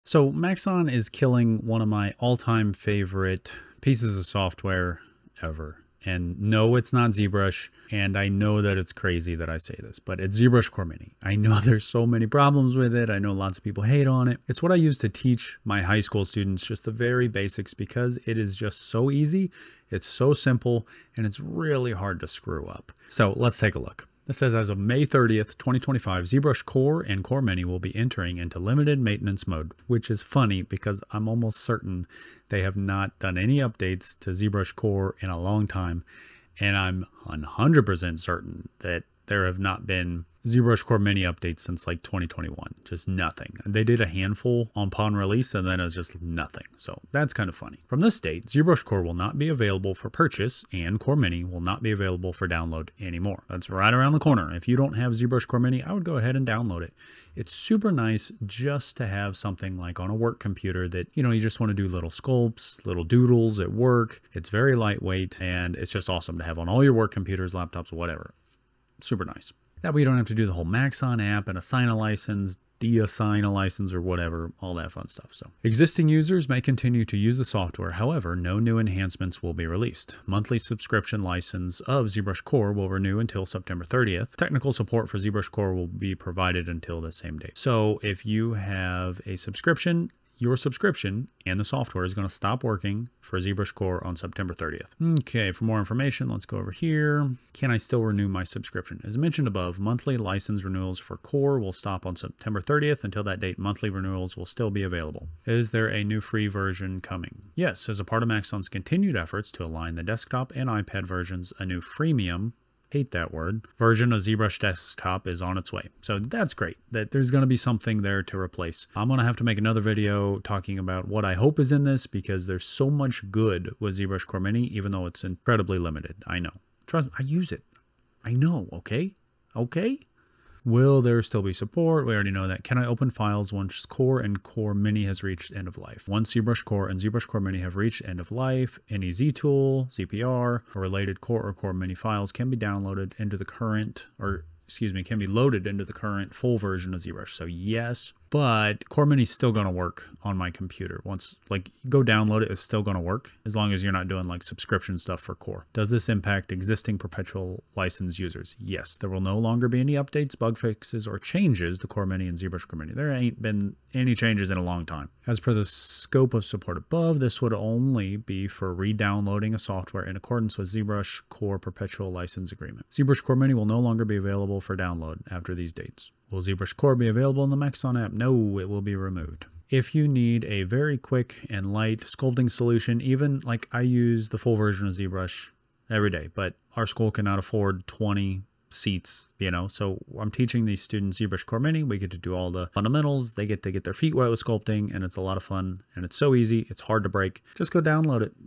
Maxon is Killing This Sculpting Software
Get it while you can... or maybe it's a good thing it's dead?